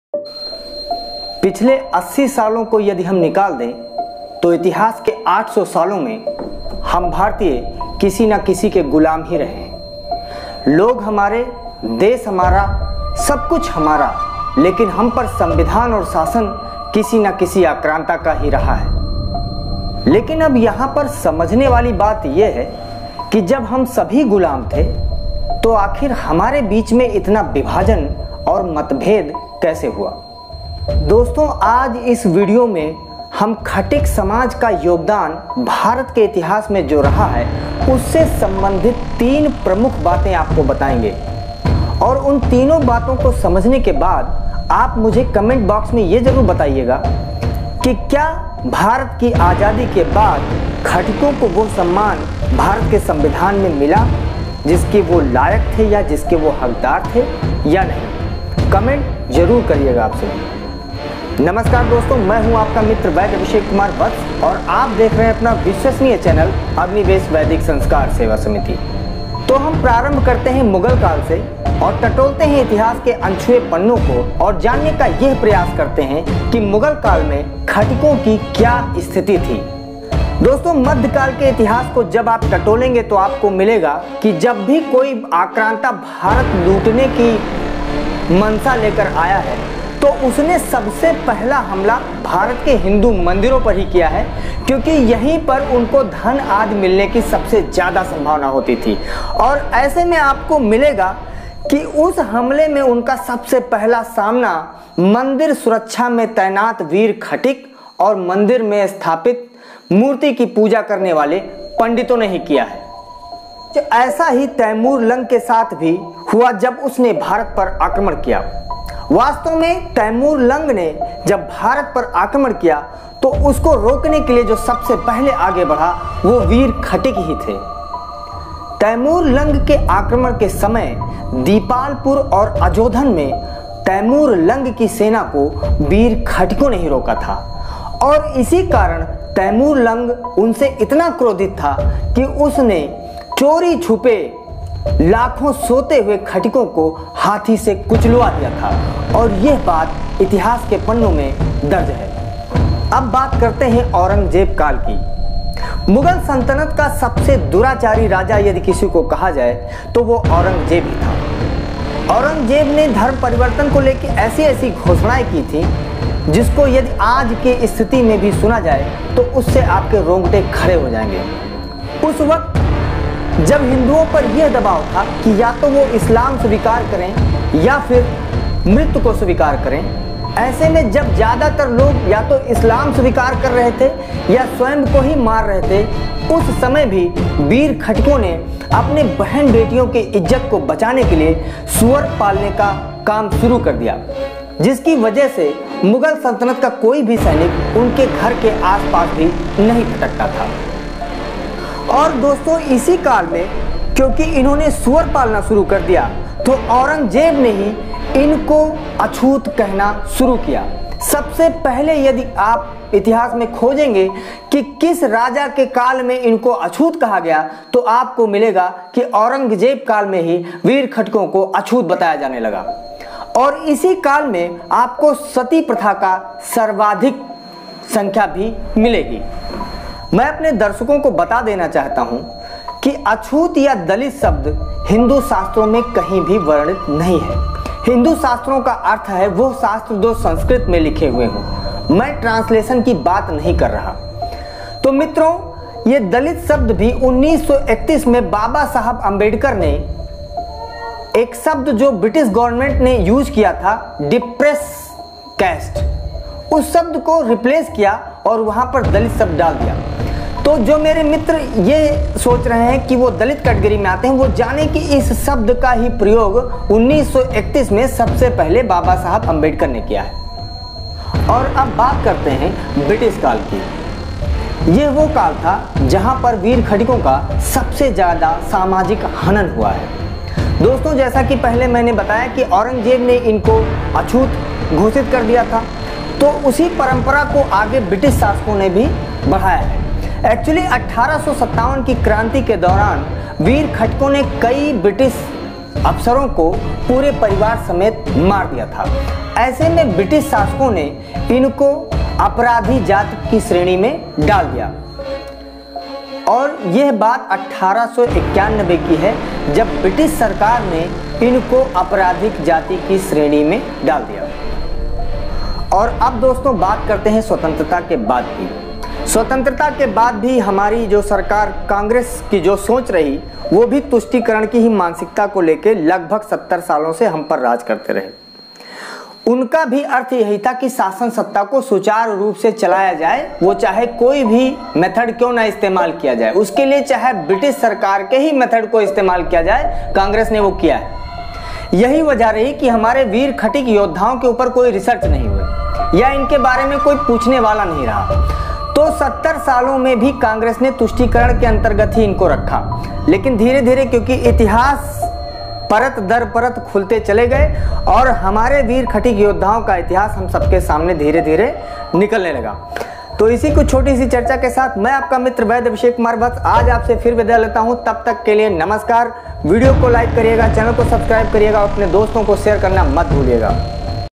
KHATIK JATI KA ITIHAAS|KAB HUA PATAN
Is video me ham khatik jati ka samay samay par kya sthiti rahi hai us par prakash daal rahey hai.
Aur unka pramaan sahit poori charcha bhi kar rahey hain.
Khatik jaati ke veeron ko itihaas me andekha kiya gaya,lekin ye vastav me hindu yoddha hain.

Instgram-vaidya abhishek kumar vatsya

This is a team work...Our team includes-
Mr.awadhesh shukla
Lallu lohara
Deepak tiwari
Deepak shukla
Ar.amitabh mishra
Alakh ram verma
Mr.atul tiwari